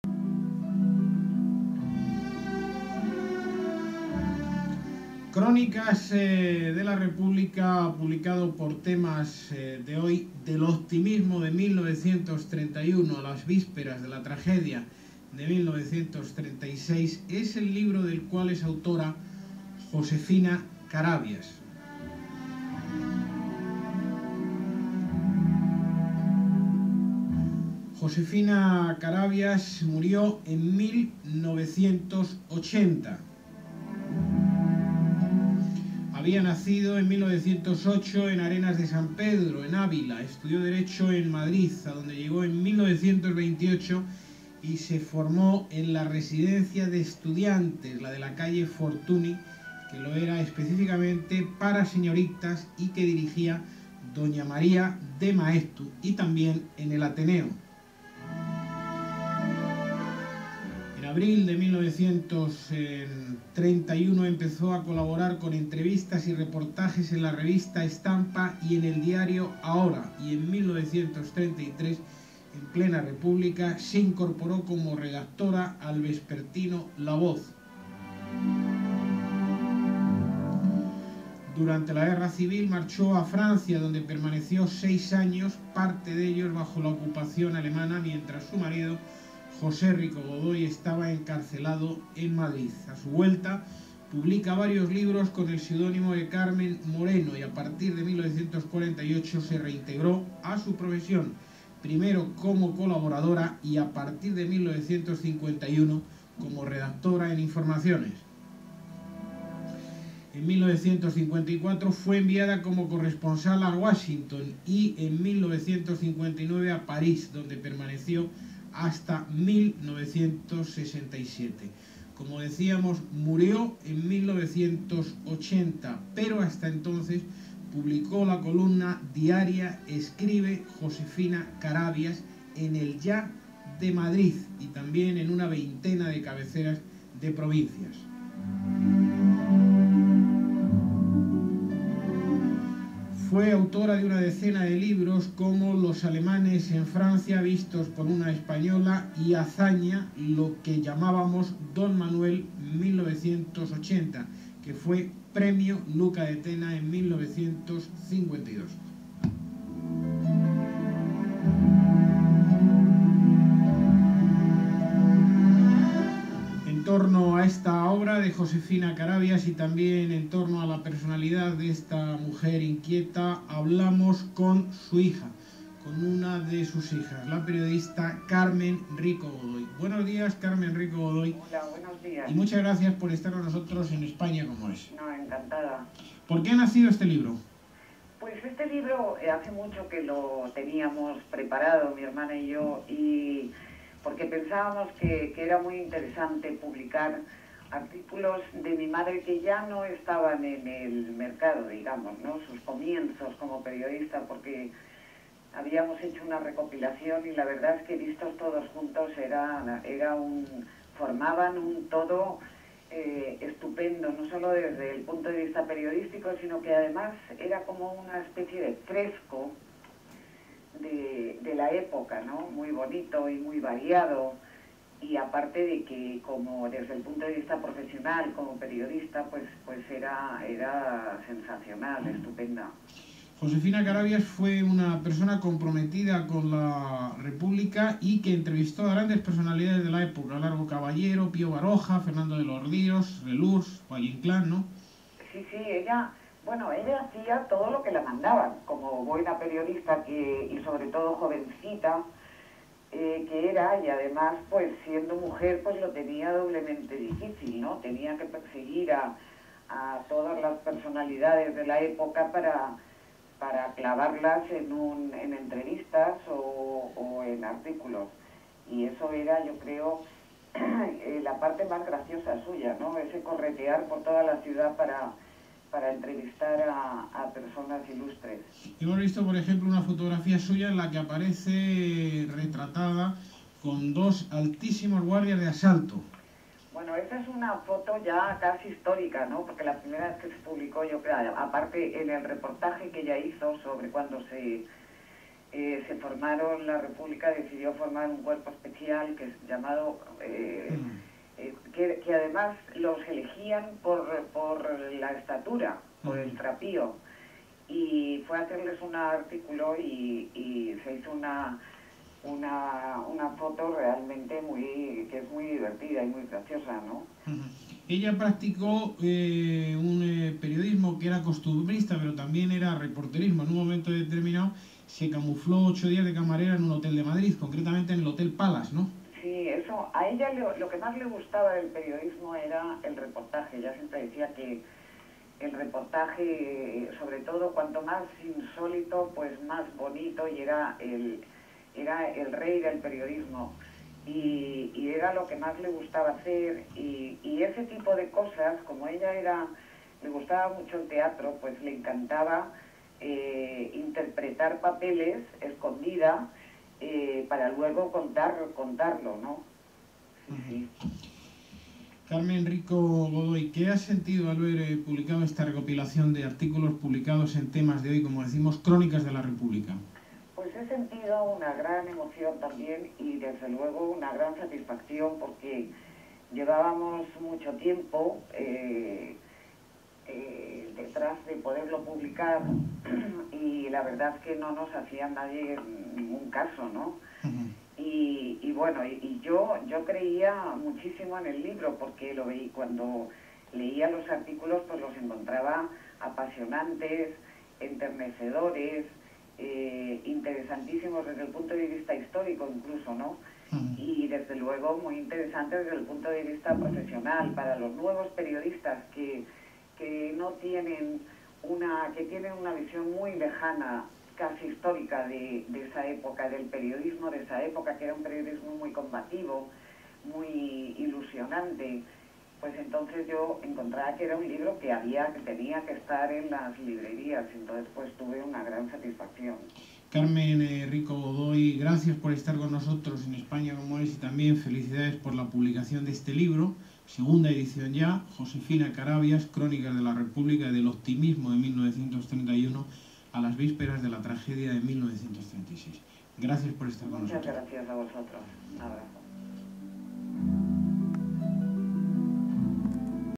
Carmen Rico-Godoy es entrevistada por Jesús Terrón.
Carmen Rico-Godoy es entrevistada por J. Terrón con motivo de la reedición del libro Crónicas de la República escrito por su madre Josefina Carabías, célebre periodista.
Carmen Rico-Godoy fue también periodista y autora de novelas adaptadas para el cine.